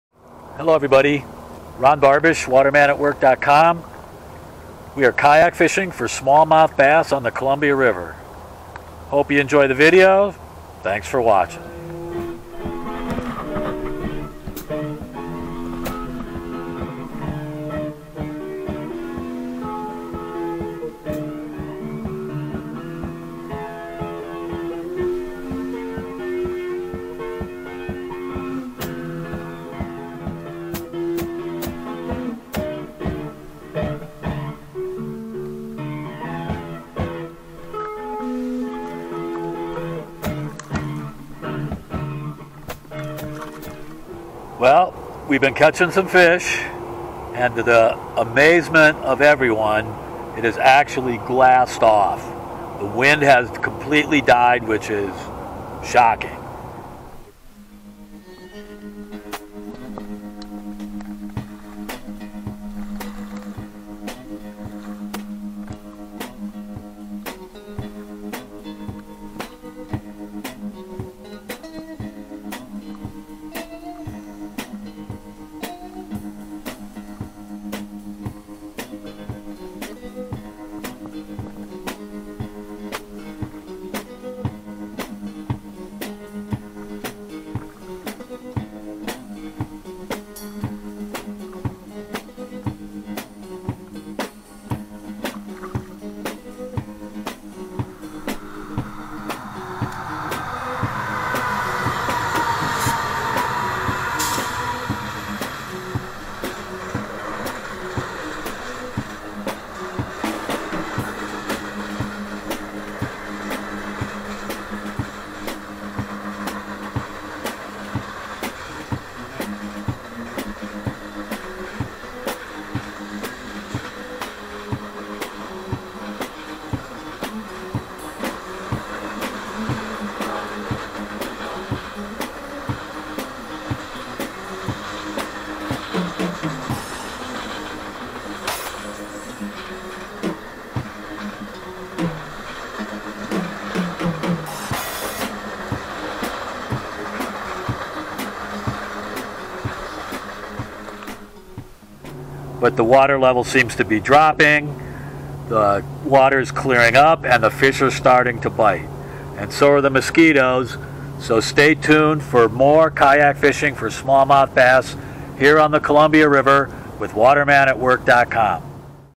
Kayak Fishing for Smallmouth Bass on the Columbia River 6-6-18
A great day of kayak fishing for smallmouth bass on the Columbia River. River conditions are getting better and so is the fishing.